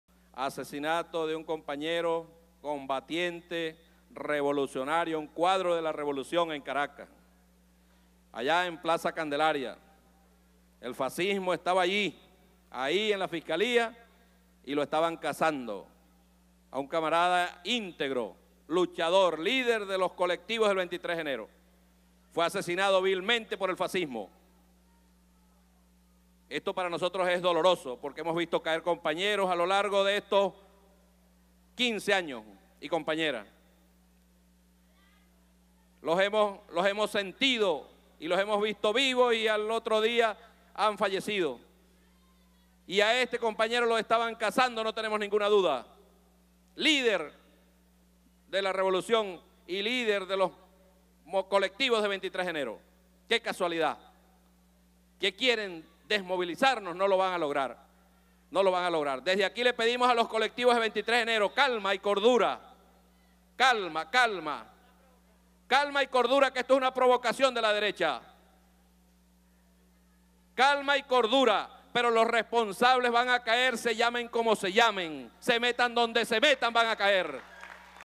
'Responsibles for murder of 23 of January leader shall fall': Diosdado
The President of Venezuela's National Assembly, Diosdado Cabello, stated that those who are responsible for the murder of a social activist belonging to the 23 of January collective during the opposition's student protests shall fall no matter who they are and where they are. teleSUR